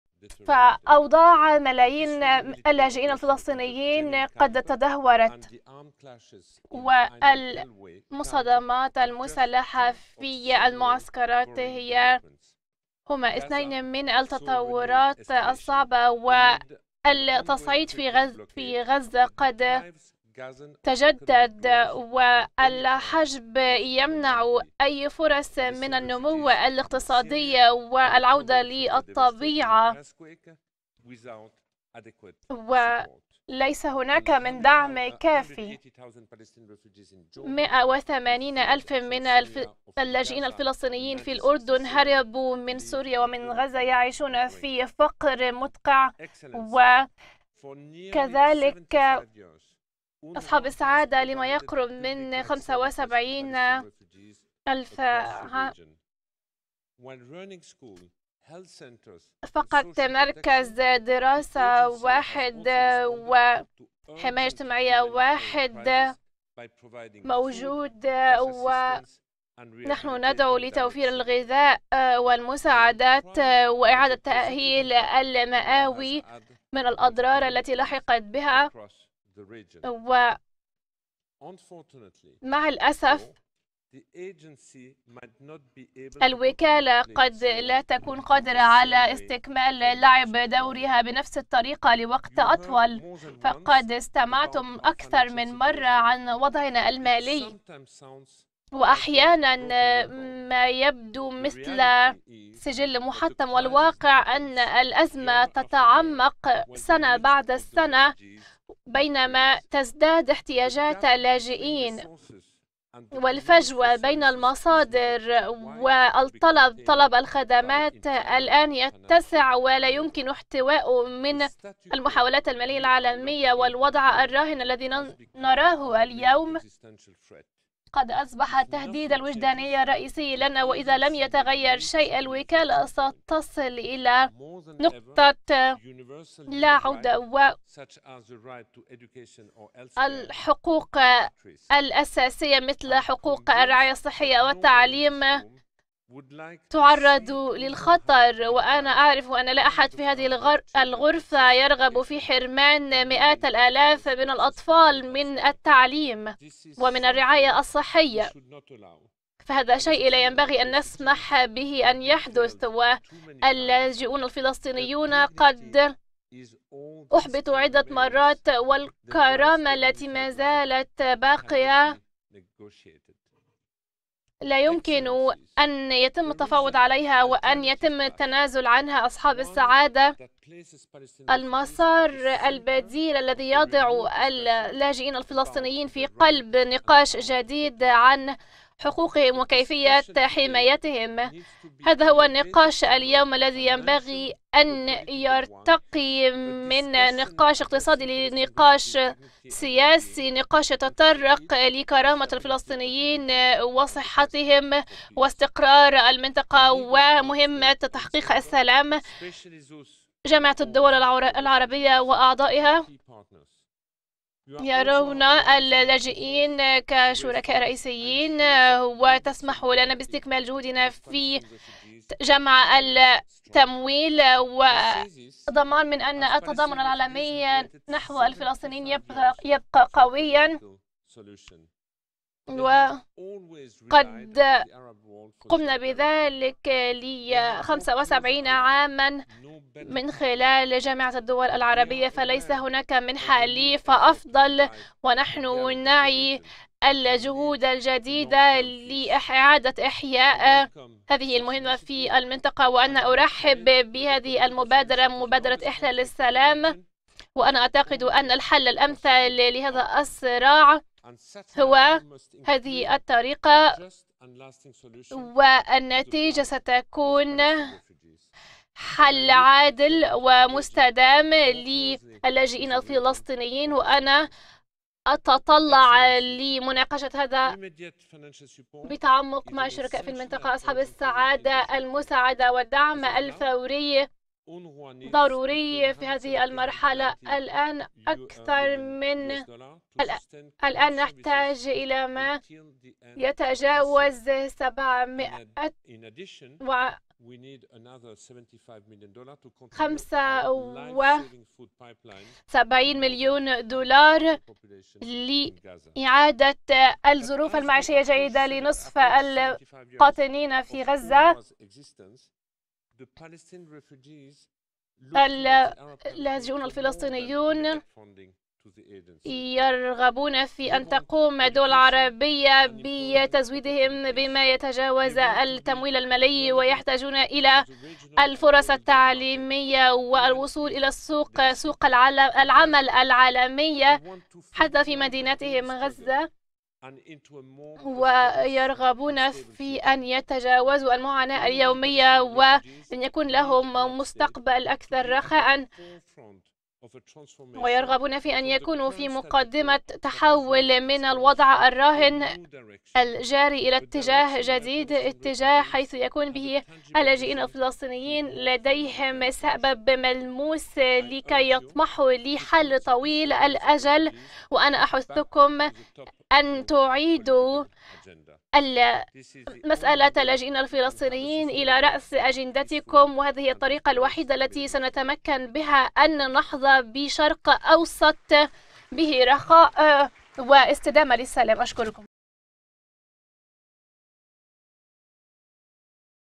لازاريني: ليس هناك دعم كاف للاجئين الفلسطينيين.. ويجب العمل على حل عادل ومستدام لقضيتهم
كلمة فيليب لازاريني - وكيل الأمين العام للأمم المتحدة خلال اجتماع وزراء الخارجية العرب بمقر الأمانة العامة للجامعة العربية في القاهرة برئاسة ناصر بوريطة وزيرِ الخارجية المغربي، لبحث عدد من الموضوعات الدائمة على جدول أعمال دورتهم العادية 160.